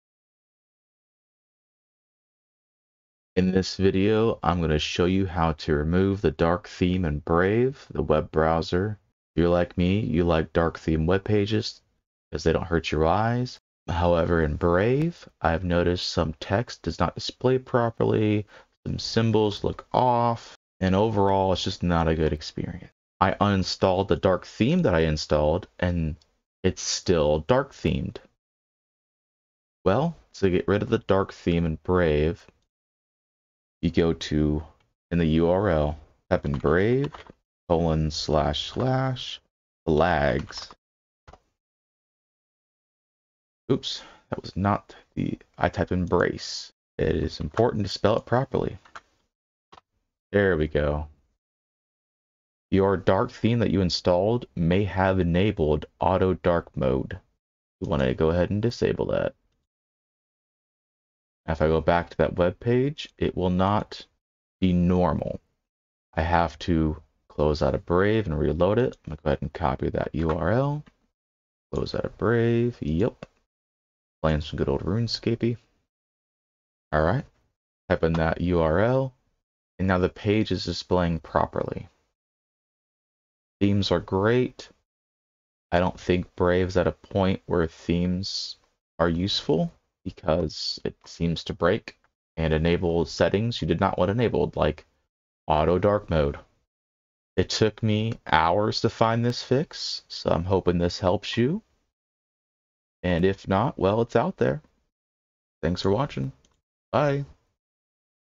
How to fix forced dark theme in Brave | desktop
Fix is found at 0:50 - Turning off "Auto Dark Mode for Web Contents."

In this video we disable the dark theme in the web browser Brave on Desktop. It took me hours to find the fix, so I am bringing it to you. Apparently when you install a dark theme in Brave there is a chance it will auto enforce dark mode. this causing the user to think they did not uninstall the theme right and the end user, me, goes crazy trying to fix it.

If you have a current IT issue and have found no solution, let me know and I will try my best to help you.